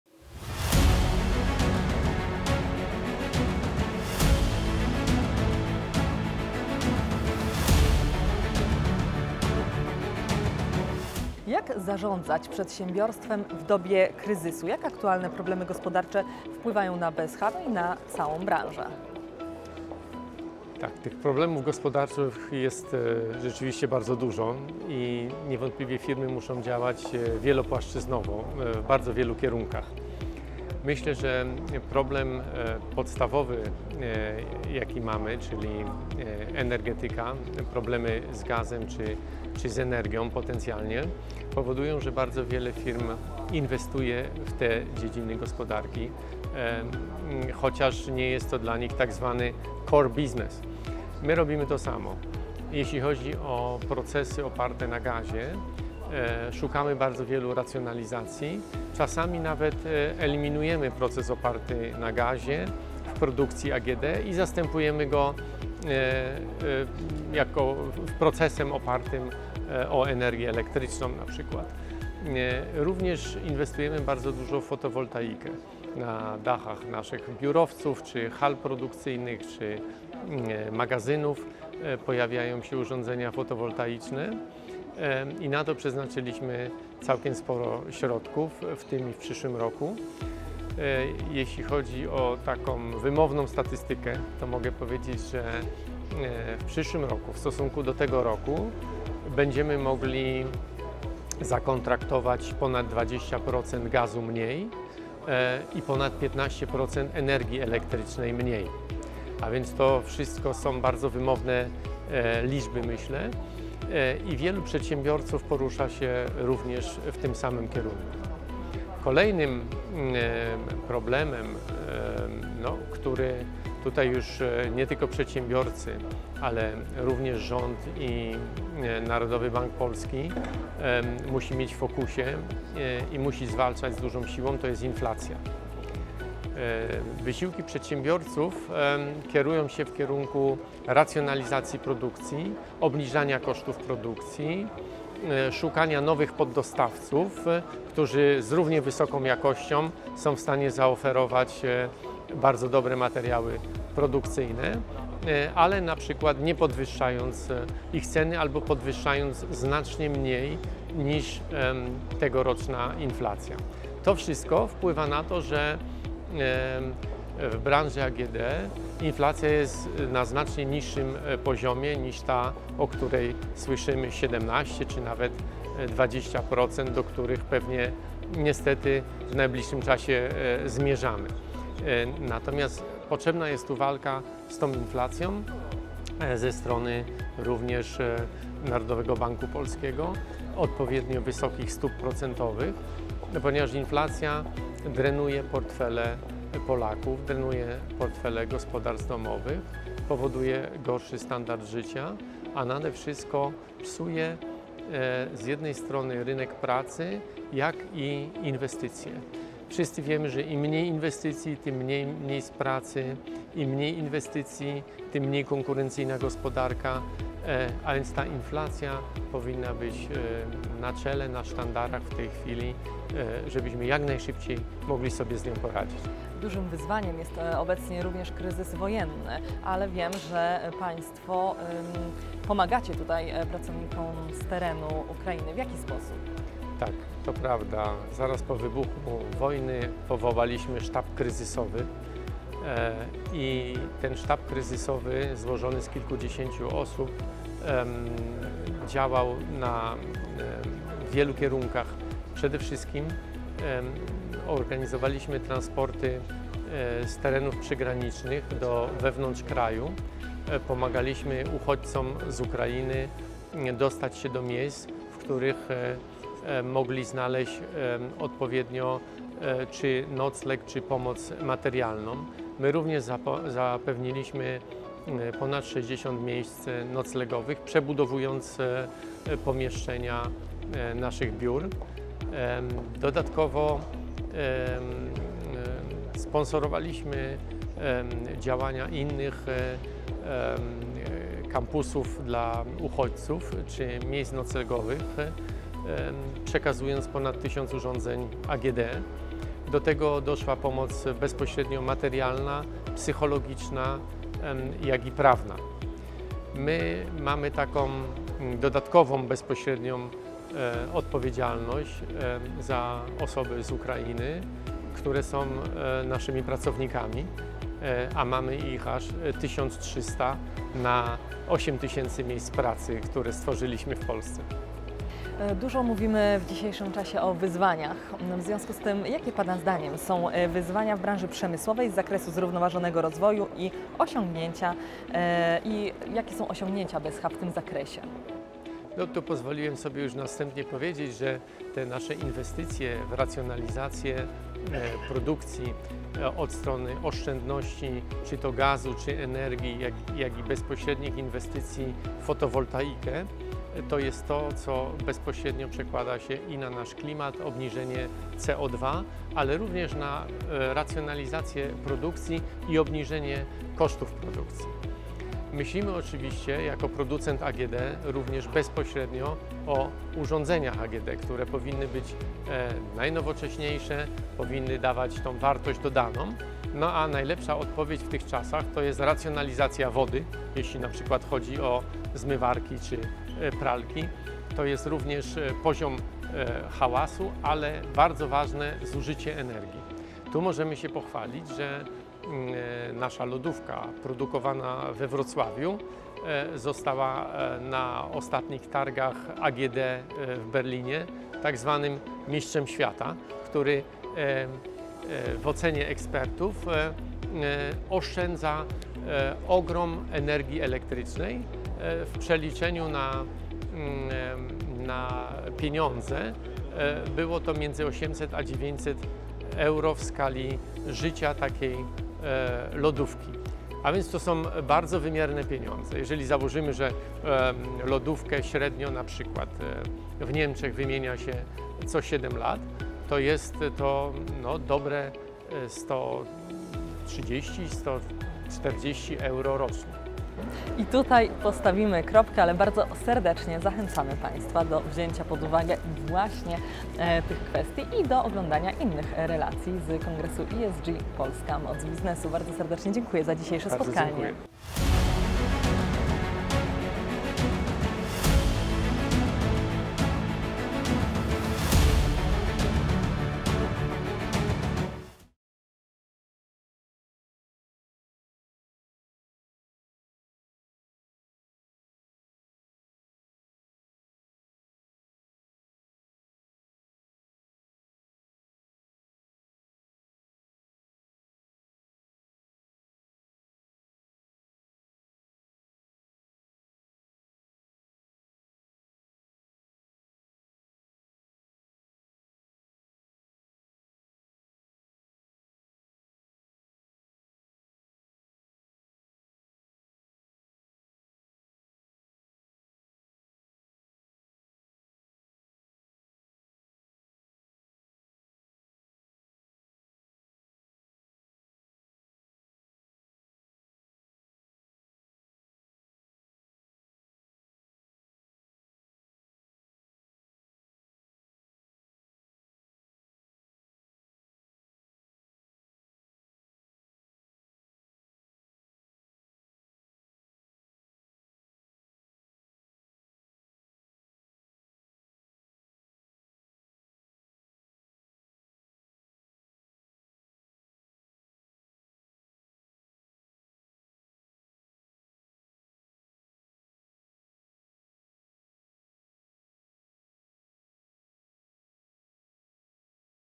Konrad Pokutycki, Prezes Zarządu BSH Sprzęt Gospodarstwa Domowego - Polska Moc Biznesu
Wywiad „gorące krzesła” wykonany podczas Kongresu ESG Polska Moc Biznesu 7 Listopada 2022 PGE Narodowy, Warszawa

W tym panel inauguracyjny:
KRYZYS I WOJNA – JAKIE WNIOSKI WYCIĄGNIE POLSKA GOSPODARKA

W którym Konrad Pokutycki brał udział.